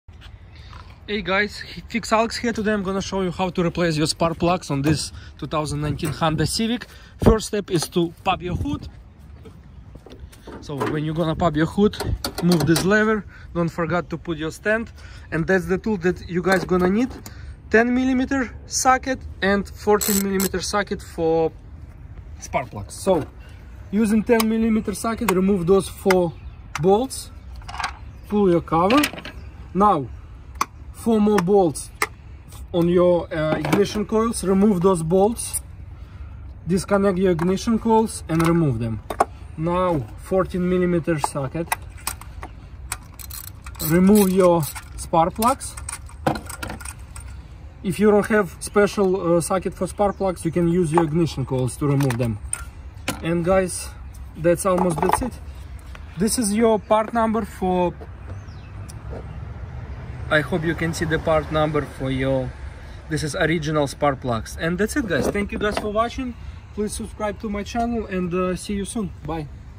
HOW TO REPLACE SPARK PLUGS ON HONDA CIVIC 16-21 REMOVE 2017 2018 2019 2020 2021
Tools 10mm and 14 mm
If You Found This Video Helpful, Please Like it.
You can also buy us a coffee!